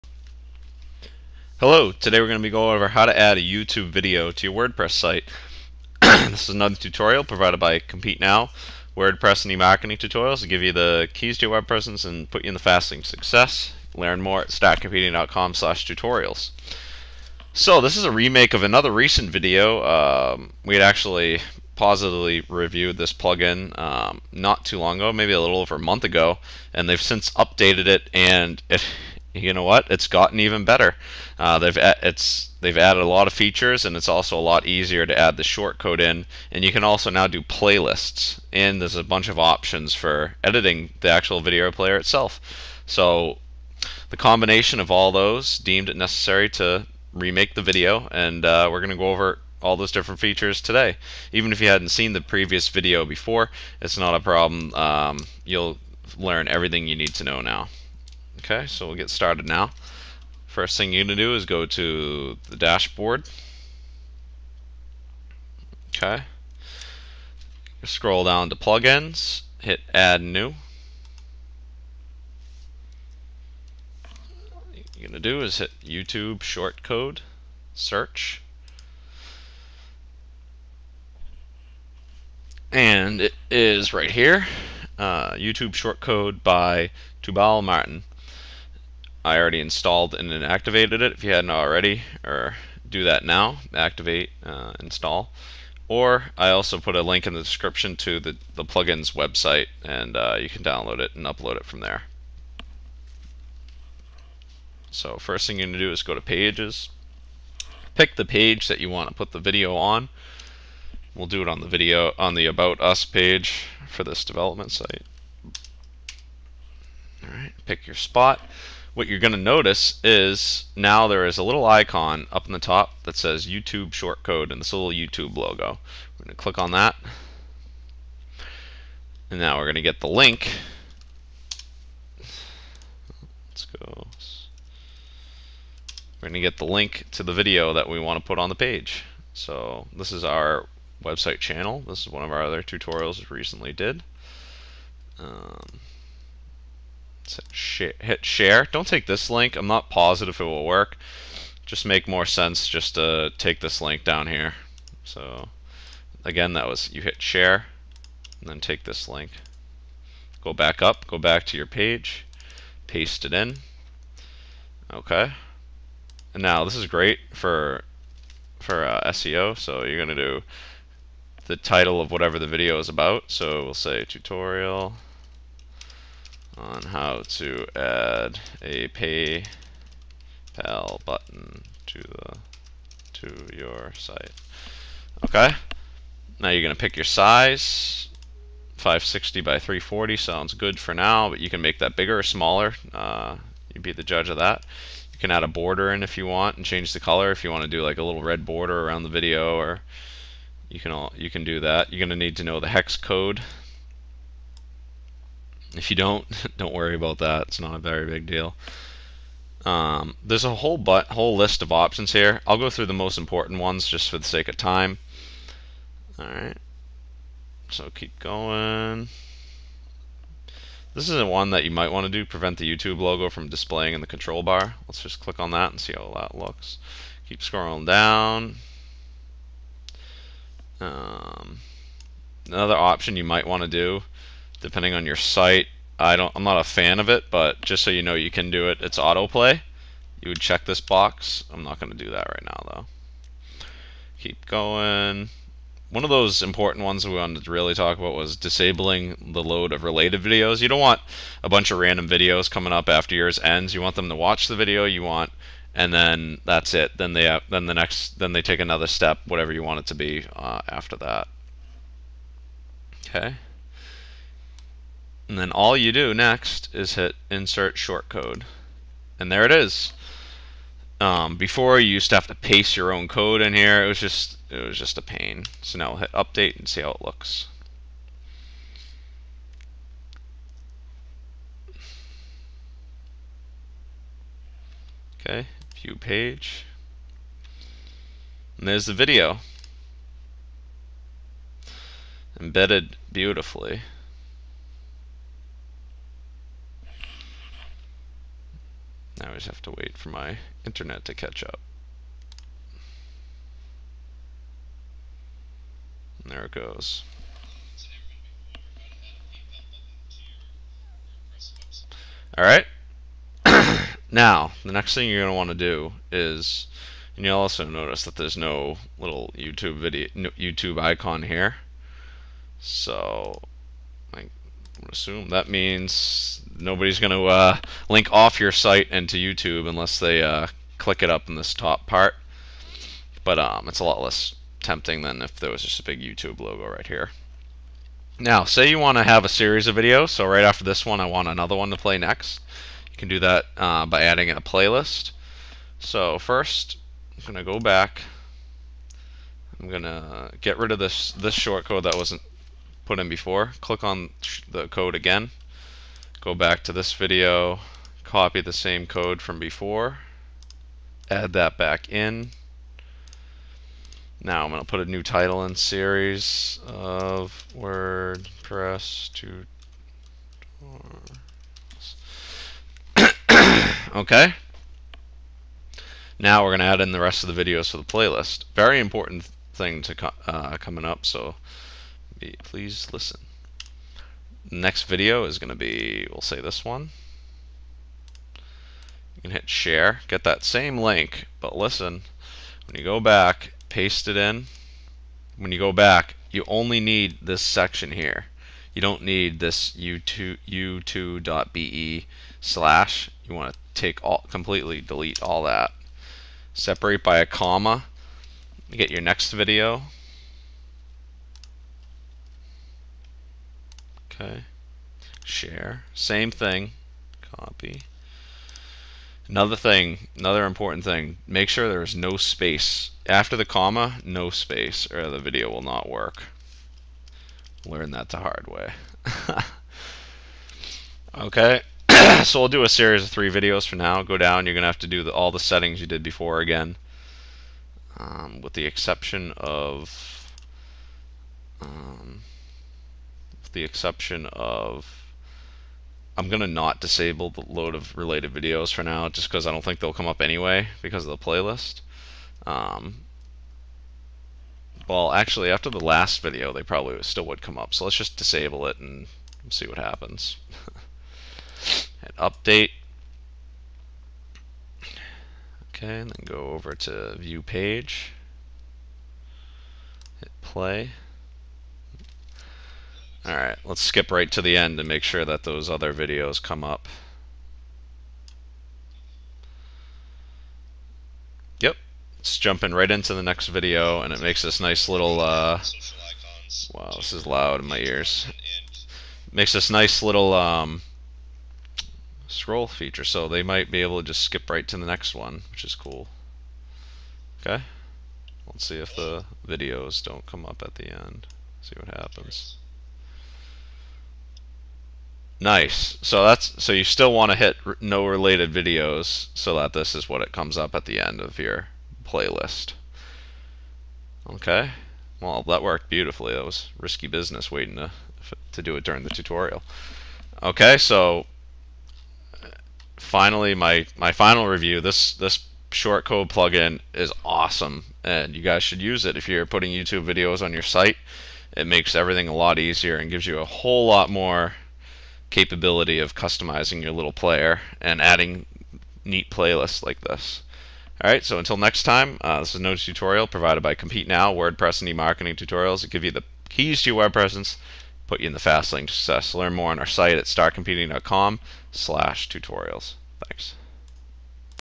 How to Easily add YouTube Videos to WordPress
In this video, I will demonstrate how to add a YouTube video to your WordPress website.  This tutorial will also demonstrate how to create a playlist and using the plugin demonstrated, will show you how to customize your player.

Pardon my Boston accent, it sometimes comes out without warning.  Enjoy.

Table of Contents:

Installing the plugin: 1:14
Next step is to pick the page and location where you want to place the video.
Inserting the shortcode: 2:00
Selecting the proper link from YouTube: 2:25
Settings walk through: 2:45
How to add a playlist: 5:43
Please watch the whole playlist tutorial, there are a few picky things to pay attention too!

To find the plugin search "YouTube Shortcode" or you can also download the plugin directly